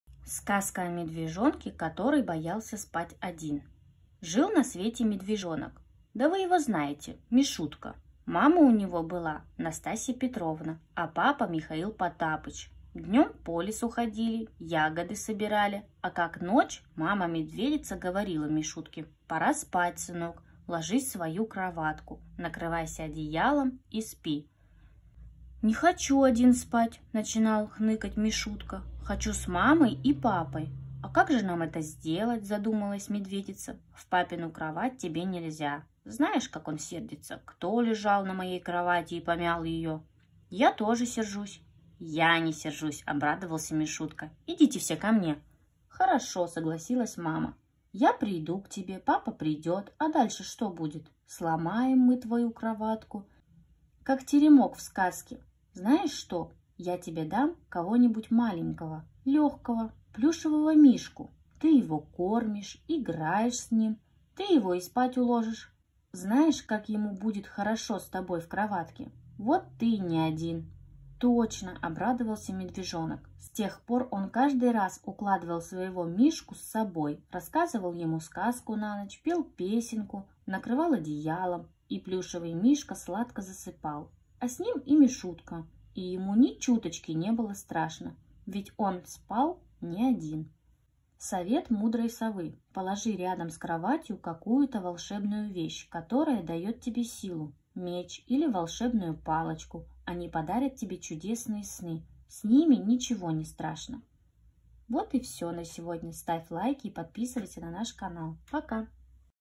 Сказка о медвежонке, который боялся спать один.
Если ваш ребёнок боится засыпать один, послушайте онлайн сказку о медвежонке 🤗